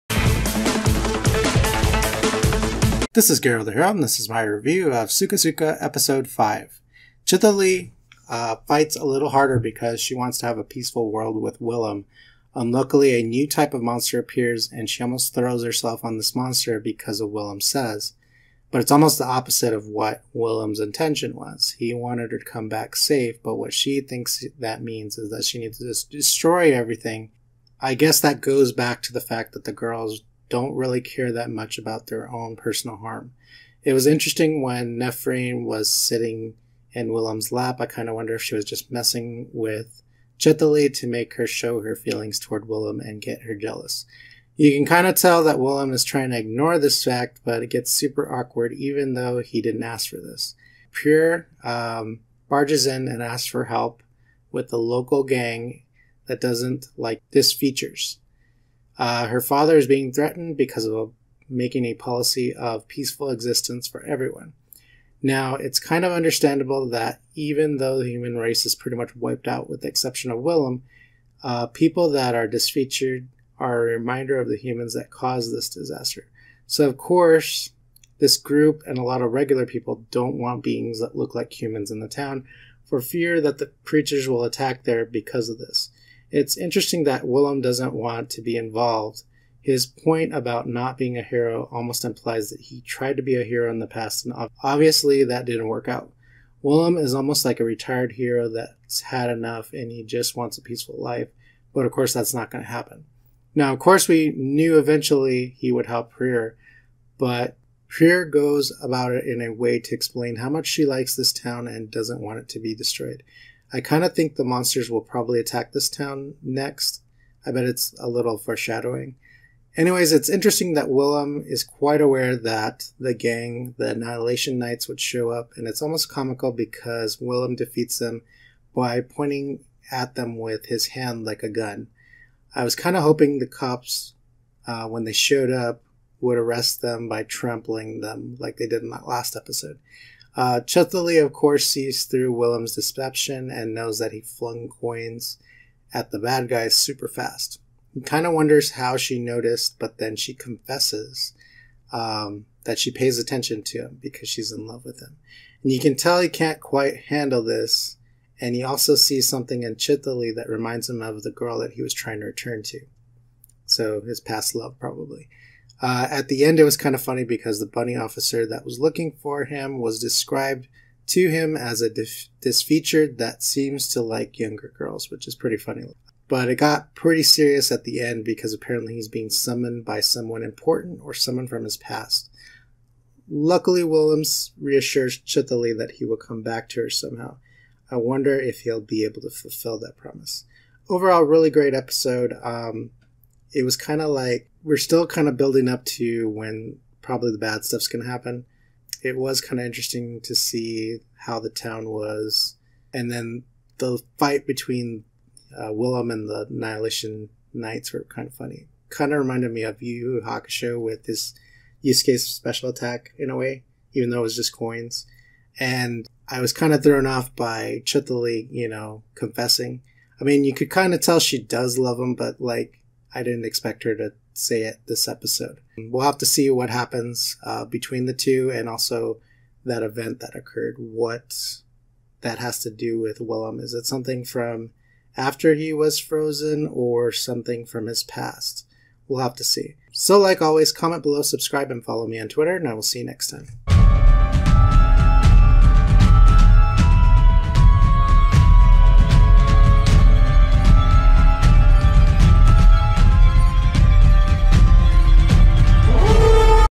SukaSuka Episode 5 Review
Chtholly pushes herself too far for a peaceful life with willem.  Later Chtholly confesses her feelings to willem and catches him off guard.